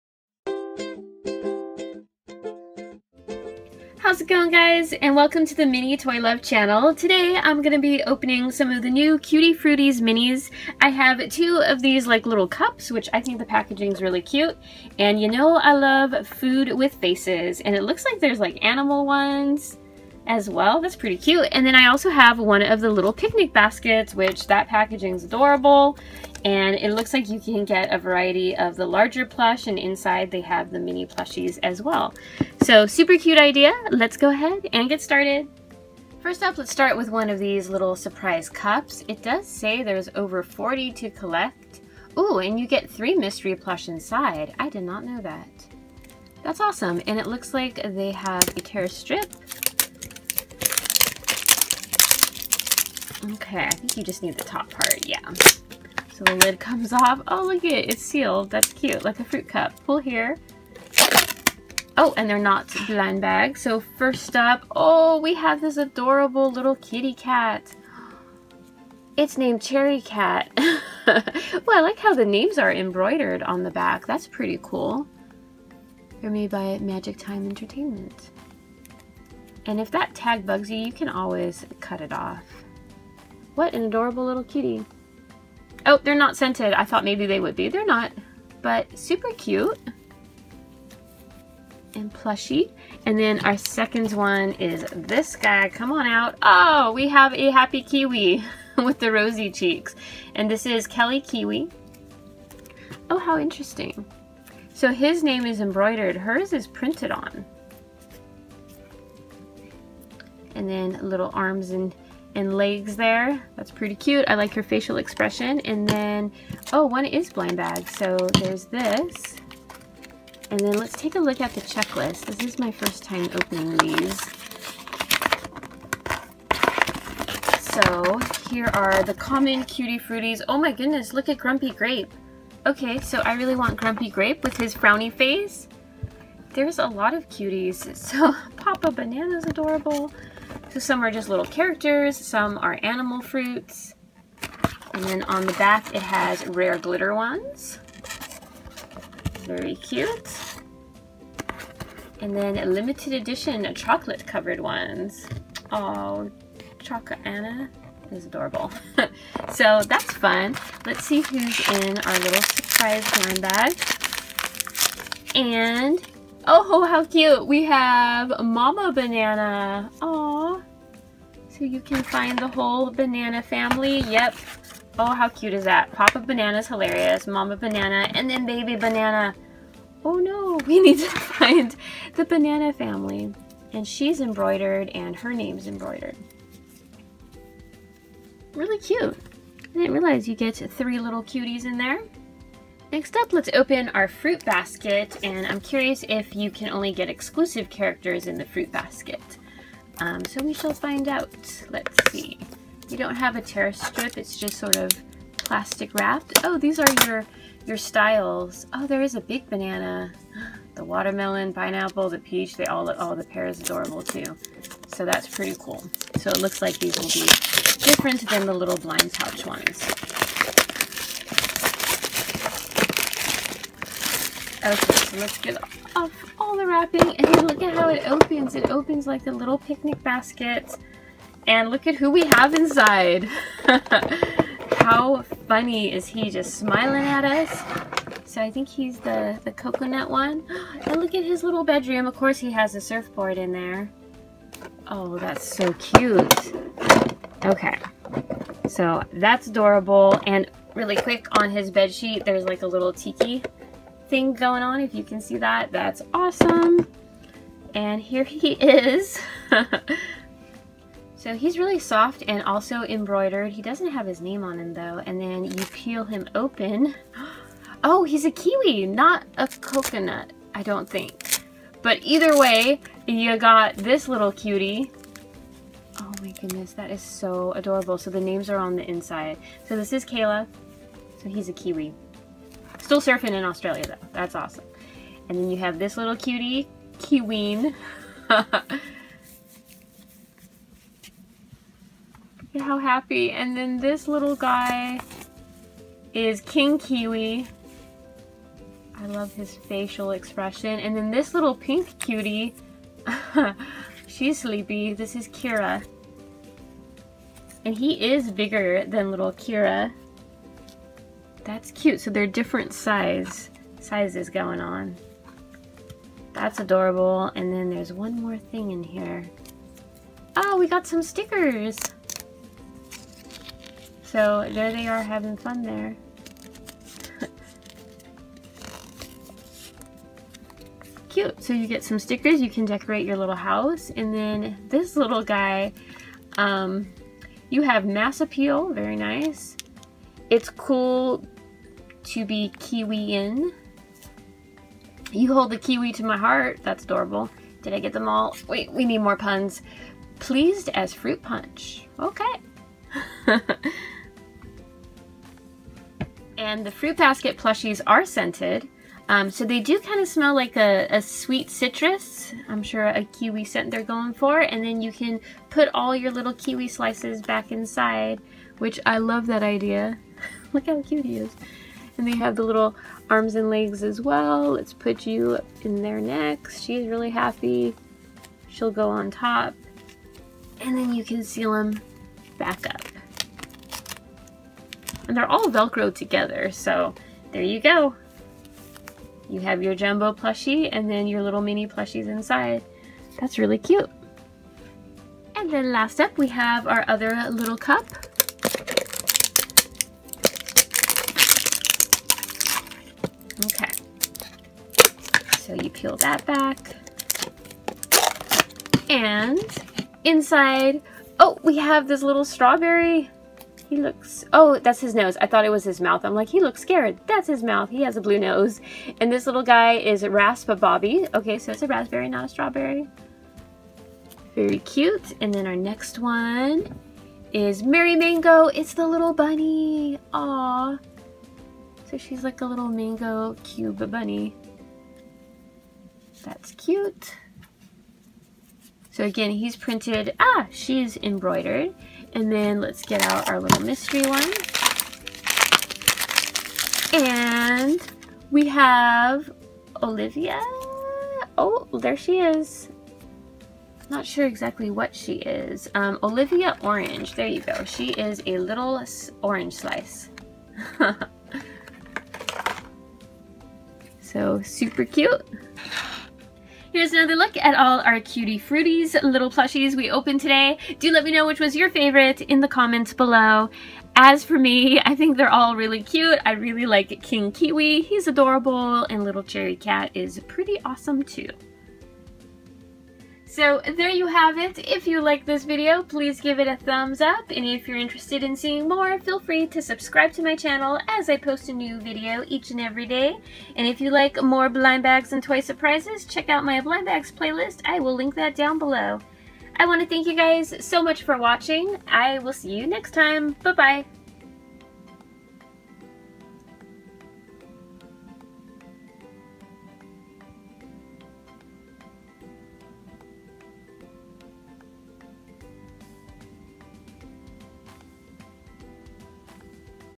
Cutie Fruities Minis Surprise Plush Blind Bags & Picnic Basket Unboxing
Today I'm opening the new Cutie Fruities Mini Plush  & Mystery Picnic Basket.